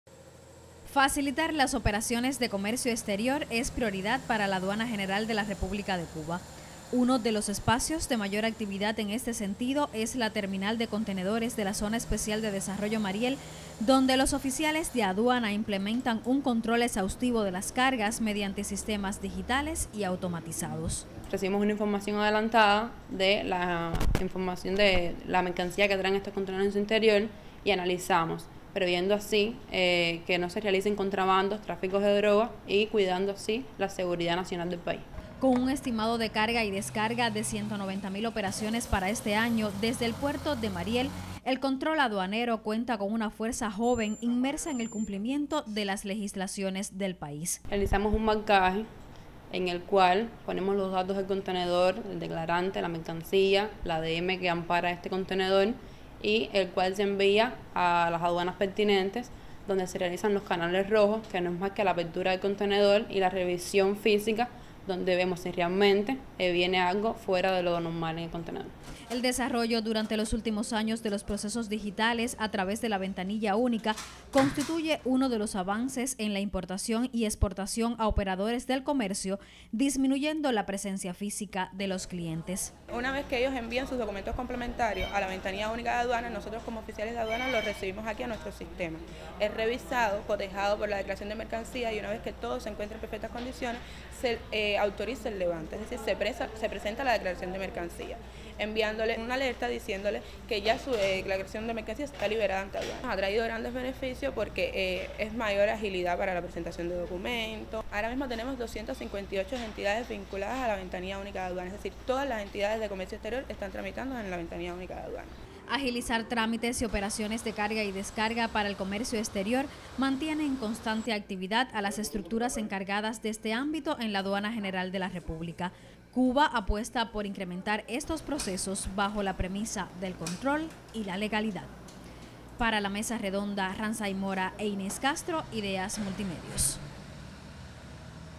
Sepa más sobre el trabajo aduanero en la importación y exportación en Cuba
Para facilitar los procesos de control aduanero vinculados al comercio exterior se prioriza la viabilización de trámites por vía digital y las operaciones de control de carga y descarga en la terminal de contenedores de la Zona Especial de Desarrollo Mariel.